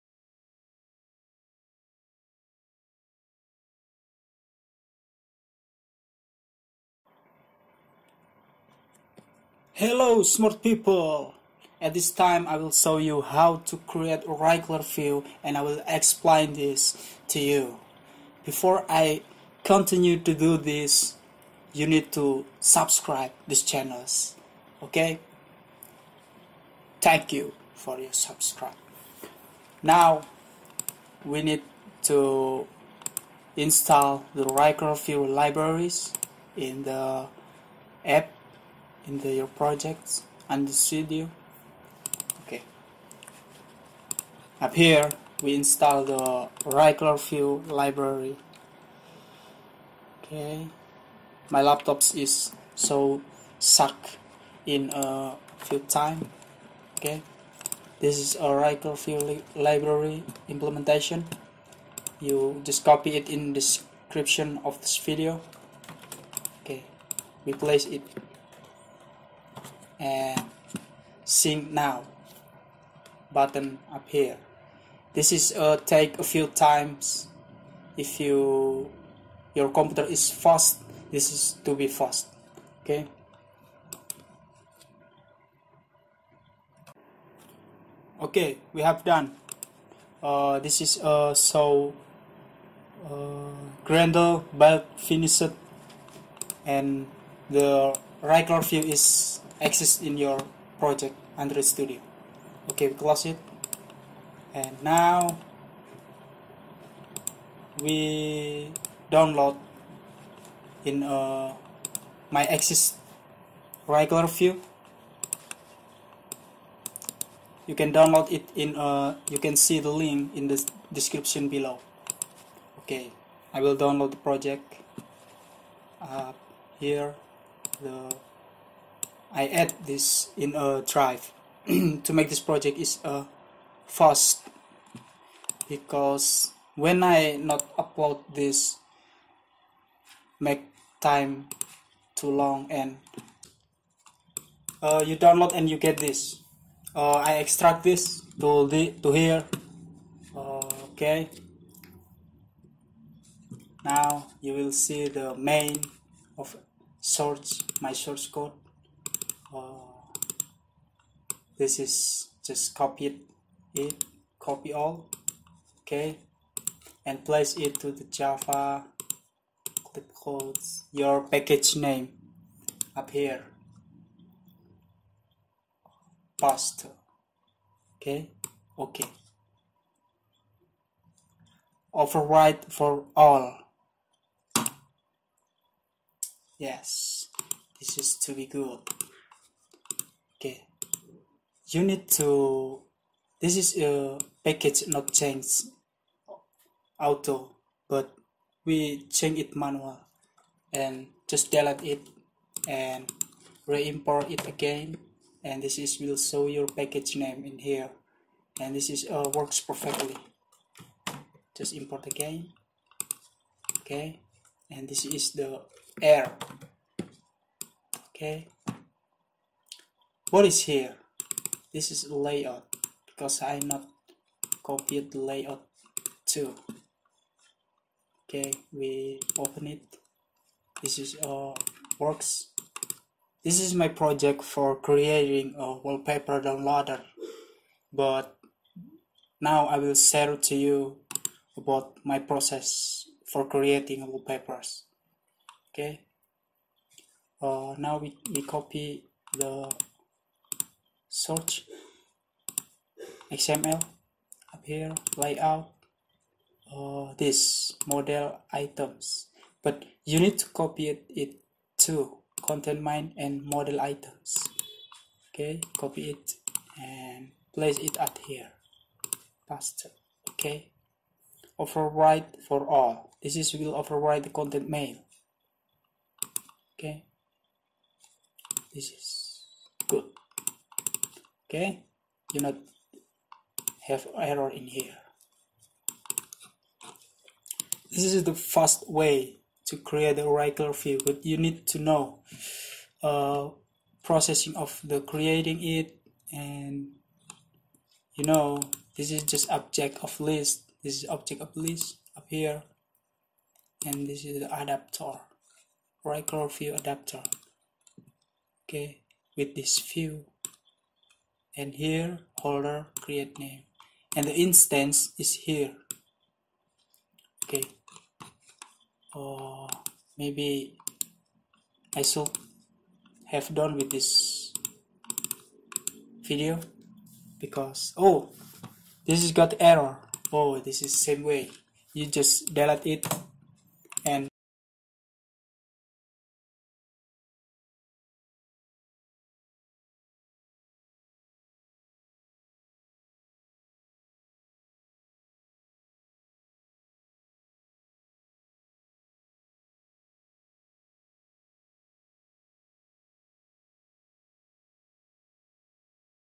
Fast Way Create Grid Recyclerview Android Studio
Hello, Here i show you creating Fast Way Create Grid  Recyclerview Android Studio.

LIBRARY HERE :
implementation 'com.android.support:recyclerview-v7:27.1.1'